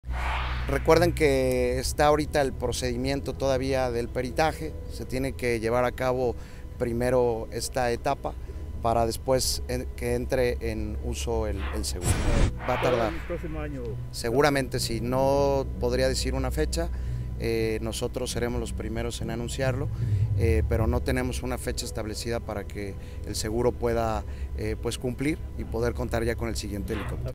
Va para largo reposición del helicóptero “Aguila 1"
* Deberá concluir investigación del accidente antes que compañía de seguros entregue uno nuevo
* Por lo pronto se cuenta ya con “Fuerza 1” y “Halcón 1”, éste último propiedad del Municipio

Aguascalientes, Ags. 26 diciembre 2022.- La reposición del helicóptero “Águila 1”, asignado a la Policía Estatal, va para largo, debido a que la compañía aseguradora deber esperar primero al dictamen final sobre las causas del accidente sufrido por la aeronave para poder iniciar los trámites de reposición, dio a conocer el vocero del Gobierno de Aguascalientes, Kike de la Torre.
En entrevista colectiva, expuso que si bien ya la Fiscalía General de la República (FGR), ha determinado que la caída de este aparato obedeció a un accidente, autoridades de aeronáutica civil del Gobierno de la República llevan a cabo el peritaje correspondiente, mismo que debe realizarse a conciencia, por lo cual no se tiene una fecha estimada para disponer nuevo de este helicóptero.
Hay que recordar que, el pasado 17 de noviembre, la aeronave “Águila 1”, asignada a la Policía Estatal de Aguascalientes, sufrió un grave accidente, desplomándose mientras el Secretario de Seguridad Pública, Porfirio Sánchez, realizaba un operativo de rutina. Tanto el funcionario policiaco como otros cuatro elementos, entre ellos el piloto, perdieron la vida al instante.
Al momento, se ha integrado a labores de vigilancia y transporte de lesionados un nuevo helicóptero, denominado “Fuerza 1”, que opera alteradamente con el “Halcón 1” de la Policía Municipal.